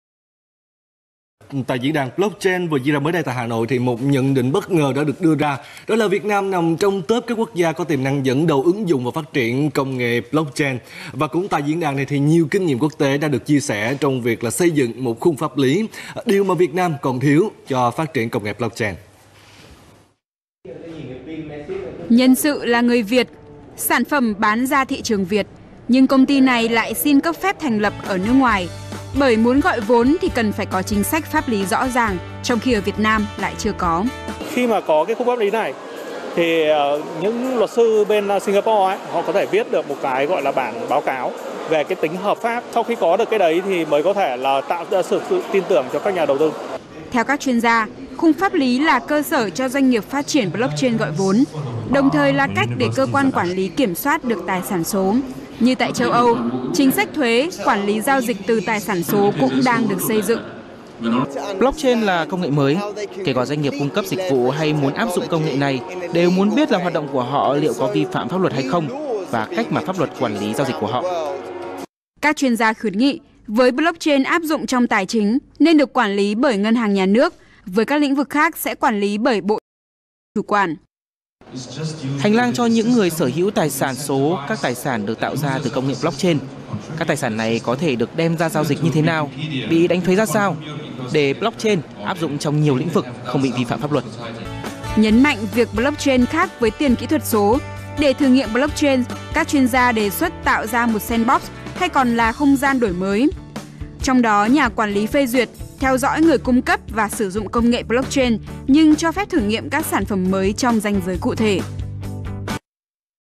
Việt Nam cần xây dựng chính sách để thị trường blockchain phát triển VTV VN Google Chrome 6 16 2
Việt Nam cần xây dựng chính sách để thị trường blockchain phát triển
(VTV1 15/6/2018)